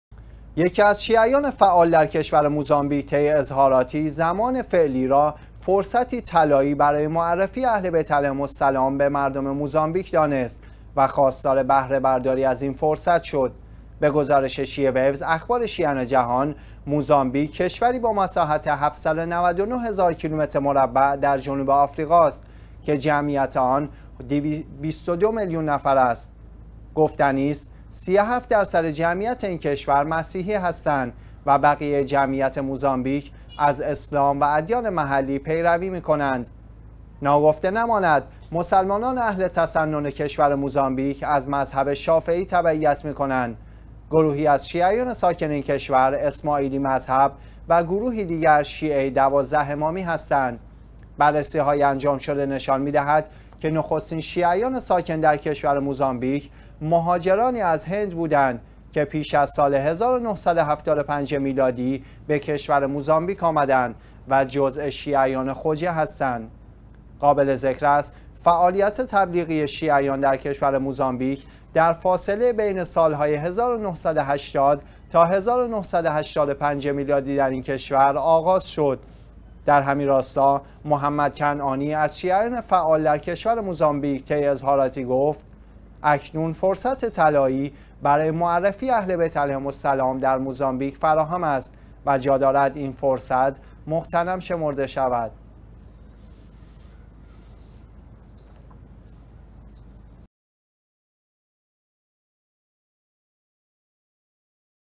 فعال شیعه موزامبیکی: فرصت طلایی برای معرفی اهل ‌بیت در موزامبیک فراهم است
یکی از شیعیان فعال در کشور موزامبیک طی اظهاراتی زمان فعلی را "فرصتی طلایی برای معرفی اهل بیت علیهم السلام به مردم موزامبیک دانست و خواستار بهره برداری از این فرصت شد."
به گزارش شیعه ویوز (اخبار شیعیان جهان)؛ موزامبیک کشوری با مساحت 799 هزار کیلومتر مربع در جنوب آفریقا است که جمعیت آن حدود 22 میلیون نفر است.
گفتنی است، 37 % جمعیت این کشور مسیحی هستند و بقیه جمعیت موزامبیک از اسلام و ادیان محلی پیروی می کنند.
ناگفته نماند مسلمانان اهل تسنن کشور موزامبیک از مذهب شافعی تبعیت می‌ کنند. گروهی از شیعیان ساکن این کشور اسماعیلی مذهب و گروهی دیگر شیعه دوازده امامی هستند.
بررسی های انجام شده نشان می دهد که نخستین شیعیان ساکن در کشور موزامبیک، مهاجرانی از هند بودند که پیش از سال 1975 میلادی به کشور موزامبیک آمدند و جزء شیعیان خوجه بودند.
قابل ذکر است، فعالیت تبلیغی شیعیان در کشور موزامبیک در فاصله بین سال‌های 1980 تا 1985 میلادی در این کشور آغاز شد.
در همین راستا "محمد کنعانی" از شیعیان فعال در کشور موزامبیک طی اظهاراتی گفت: اکنون فرصت طلایی برای معرفی اهل ‌بیت علیهم السلام در موزامبیک فراهم است و جا دارد این فرصت مغتنم شمرده شود.